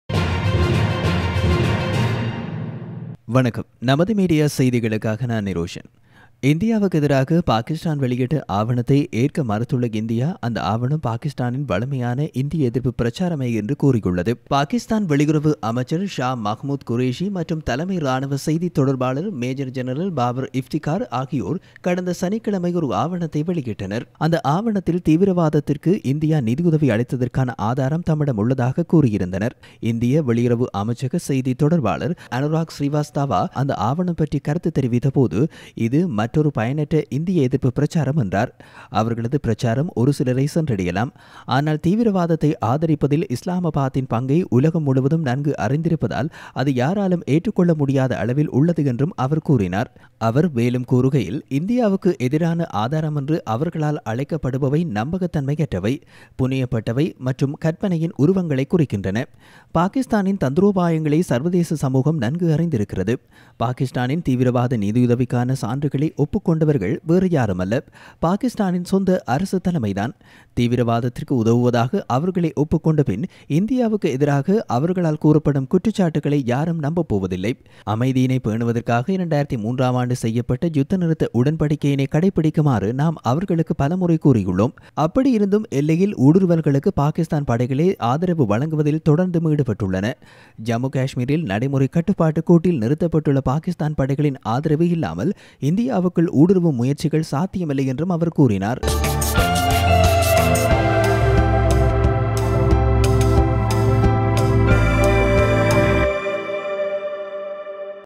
India has refused to accept Pakistan's document against India | Namathu Media World News Tamil
India has refused to accept the document issued by Pakistan against India, claiming that the document is Pakistan's usual anti-India propaganda. Pakistani Foreign Minister Shah Mahmood Qureshi and Chief Military Spokesman Babar Iftikhar released a document last Saturday. Commenting on the document, Indian Foreign Ministry spokesman Anurag Srivastava said it was another useless anti-India campaign.

Show : Namathu Media World News